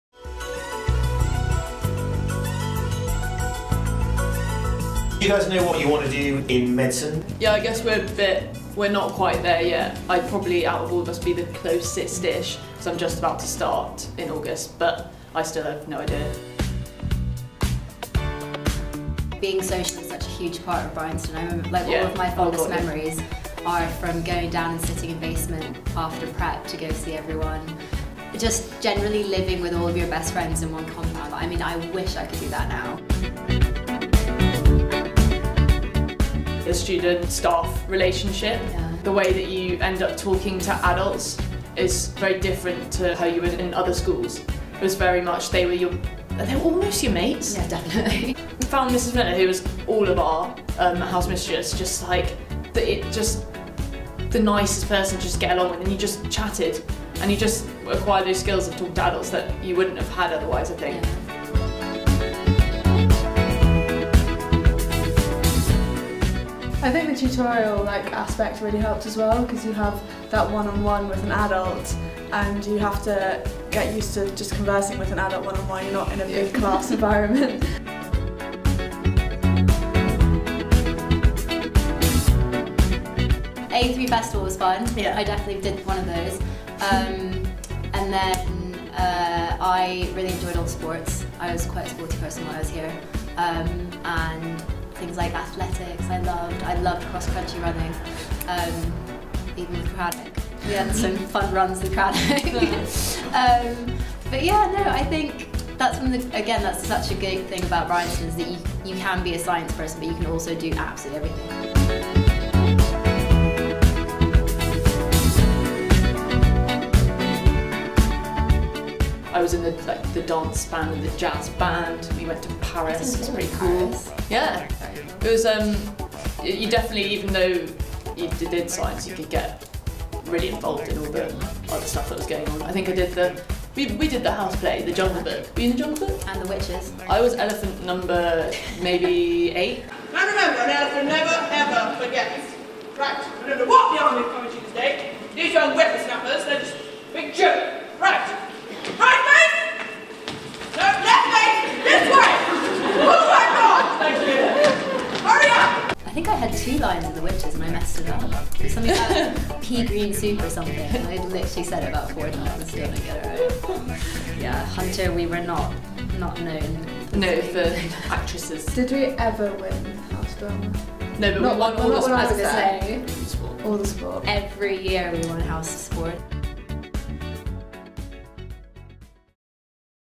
Bry90: OB Memories - The three medics from Hunter (film 33)
We speak to three OBs about their time at Bryanston! Look out for the next Bry90 film on Monday.